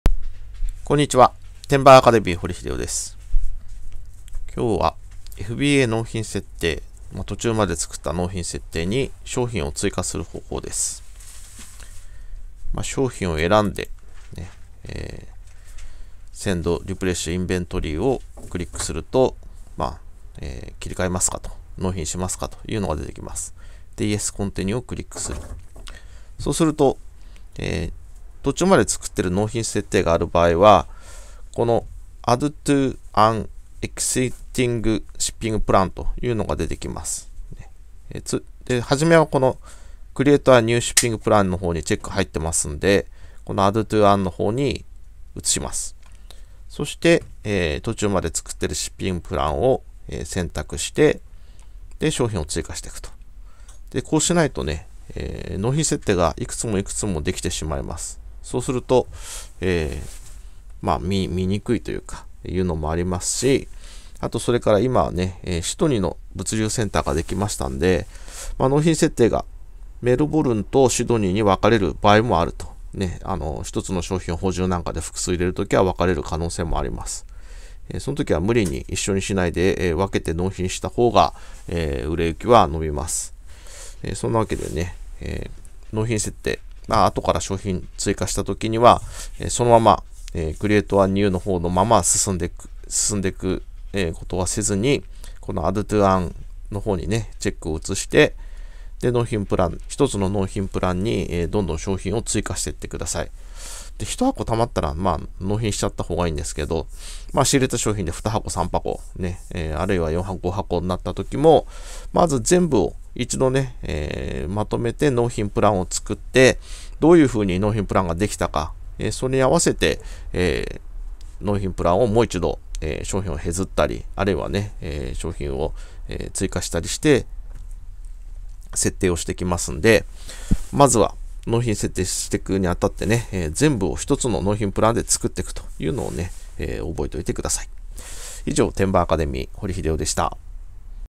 FBA納品設定の修正 商品を追加する方法
Amazon納品設定、途中まで作った納品設定に商品を追加する方法

1日10分で月10万円を稼ぐ! Amazon輸出ビジネスがわかる本　出版社:ソシム (2013/11/22)